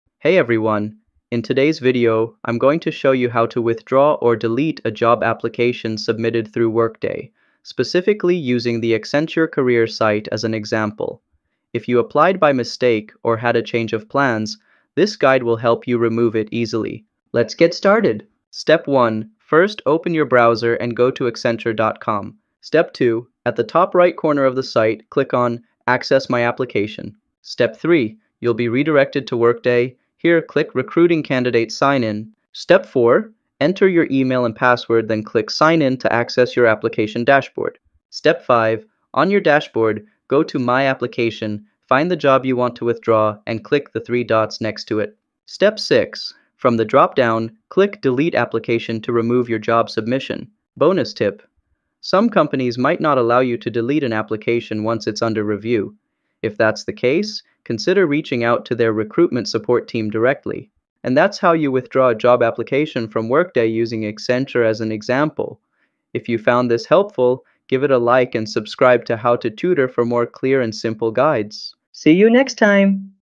How to Withdraw Application from Workday (FULL TUTORIAL 2025)
Applied for a job but changed your mind? In this 2025 step-by-step tutorial, I’ll show you exactly how to withdraw your job application from Workday, whether you're using a desktop or mobile device.

You’ll learn how to find your submitted application, withdraw it properly, and confirm that it’s been removed from the employer’s review.

✔️ Works on all Workday-powered career sites
✔️ Cancel job applications safely and quickly
✔️ Updated Workday interface for 2025